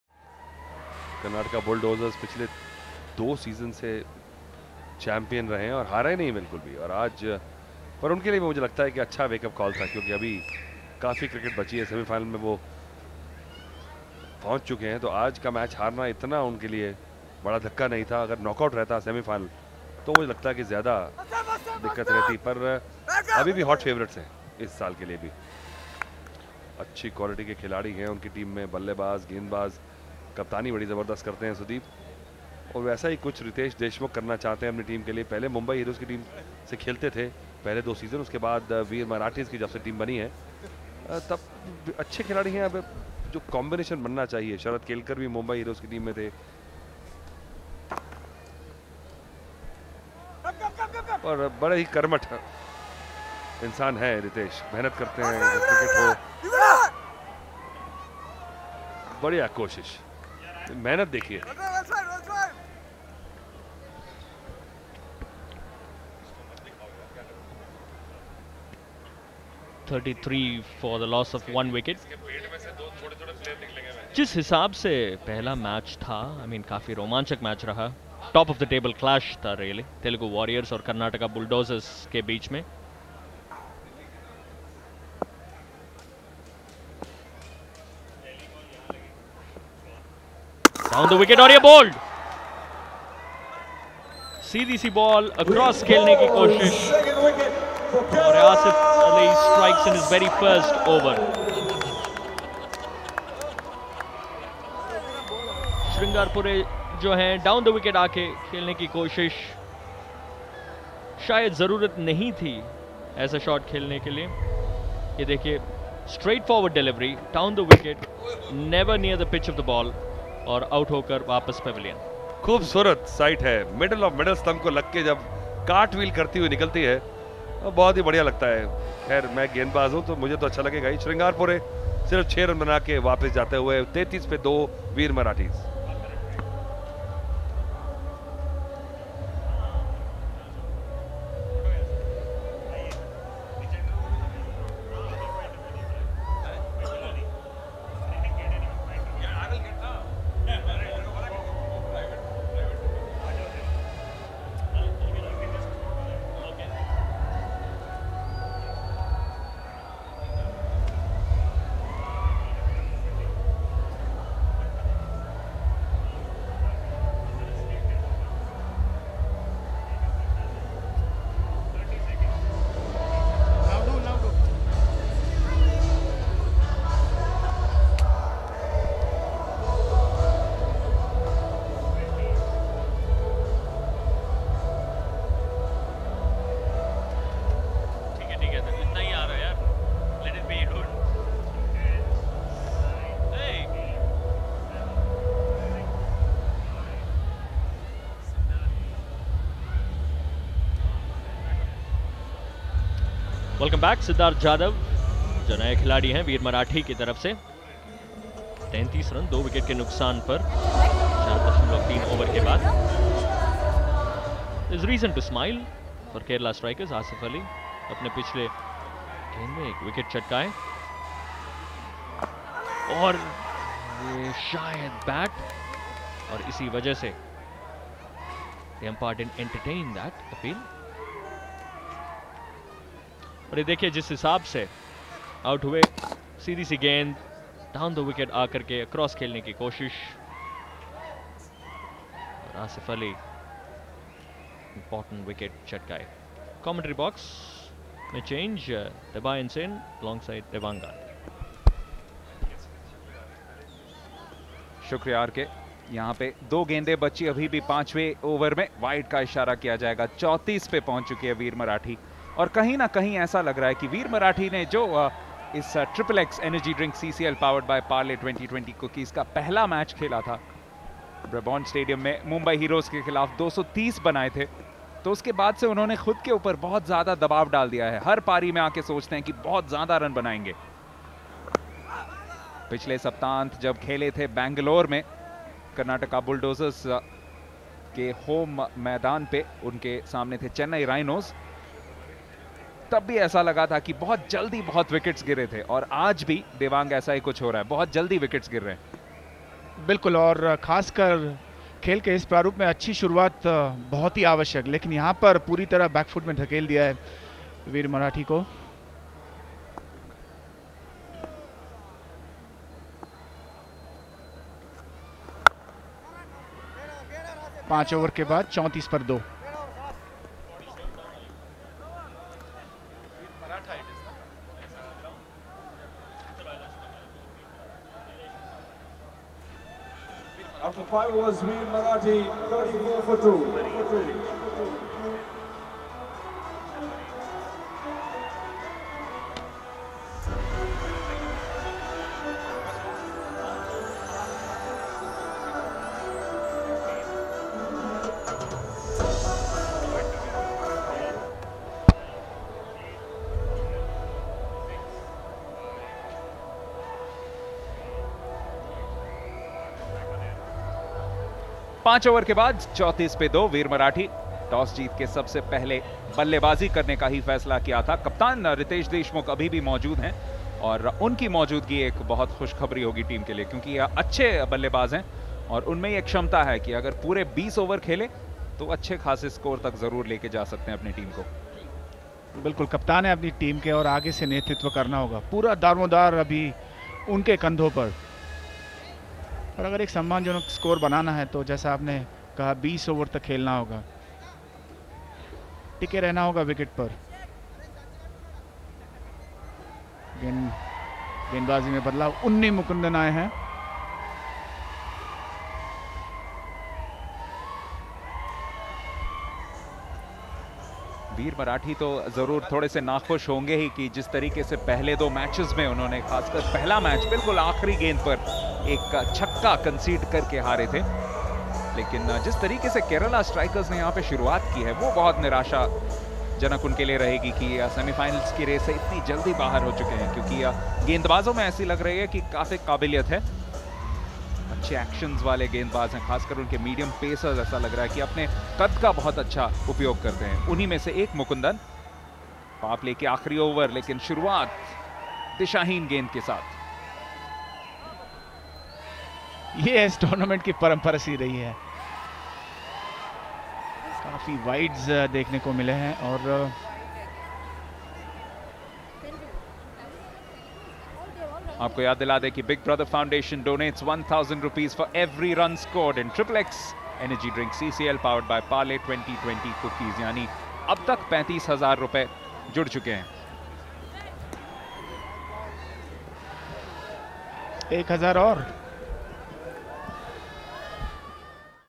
Kerala Strikers Supporters Excited After Veer Marathi Lost Wicket | Riteish Deshmukh Disappointed
Riteish Deshmukh disappointed for losing wicket by Kerala Strikers.